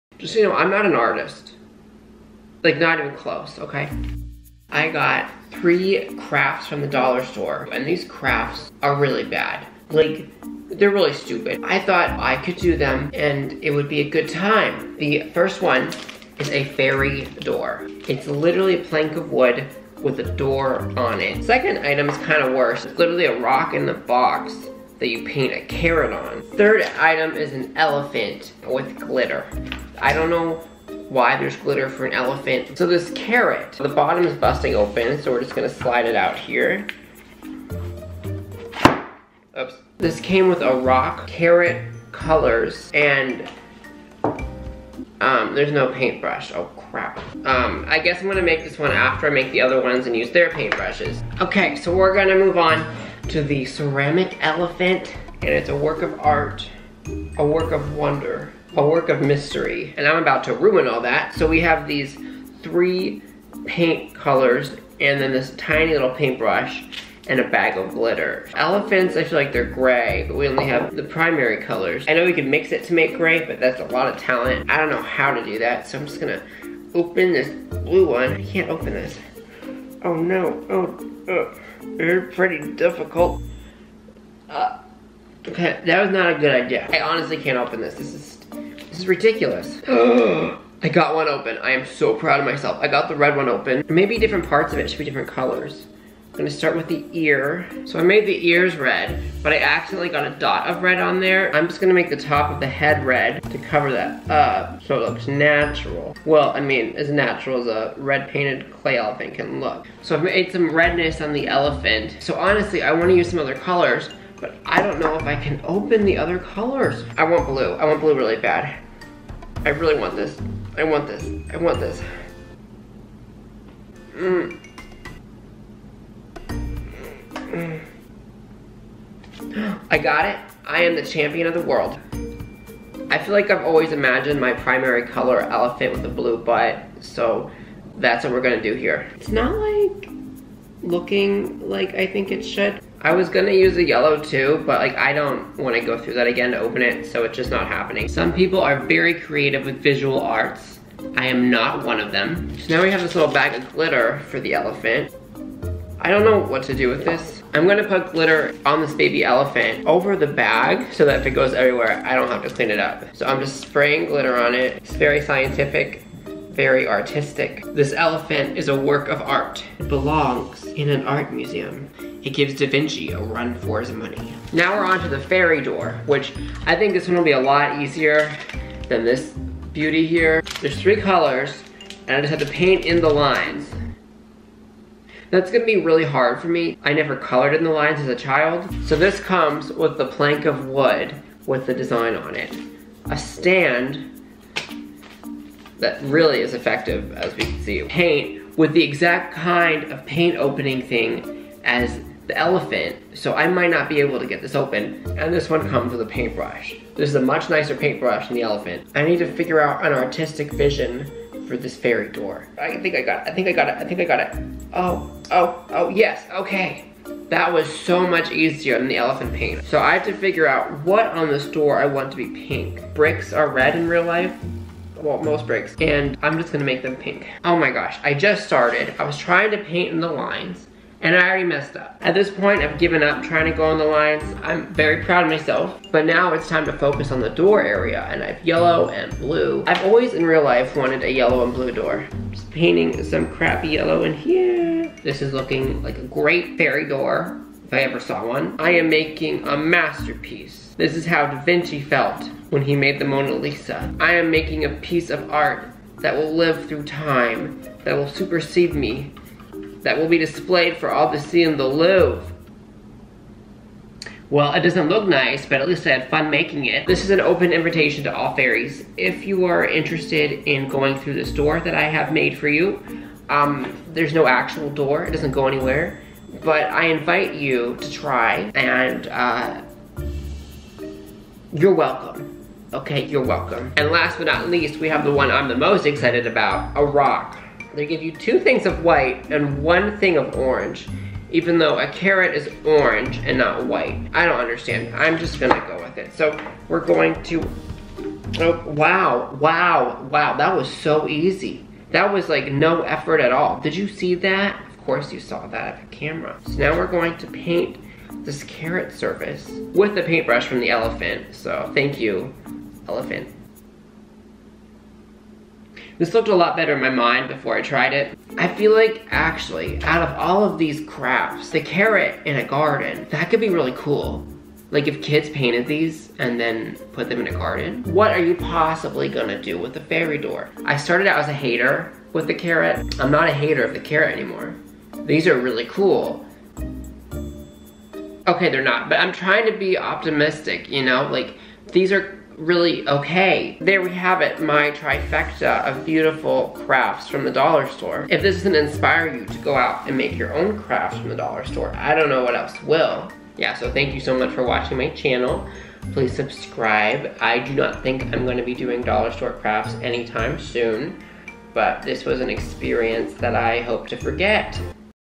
Making Weird Dollar Store Crafts
I found the three weirdest crafts from the dollar store, and made them on the internet.

Have you ever walked around the dollar store, and been like, why would anyone make that cheap, strange craft?  Well, I bought the three weirdest crafts, and made them for Youtube.

Ever wanted to buy a rock in a box, with a carrot on it that you're supposed to paint, and put in a garden?  No? Well, I found that at the dollar store, and I made it.

Ever want a fairy door in your house?  No?  Well, I bought that too, and my painting skills aren't the best.

Ever want to paint a small elephant?  Did you say YES?  But wait--did you want to put glitter on the elephant?  No?  Well, I put glitter on a terribly painted elephant, too.

Does anyone ever read this?  Please comment the word, 'Madagascar' if you've read this!

About me:

My name is Jameson Thomas, and I am a new Youtuber.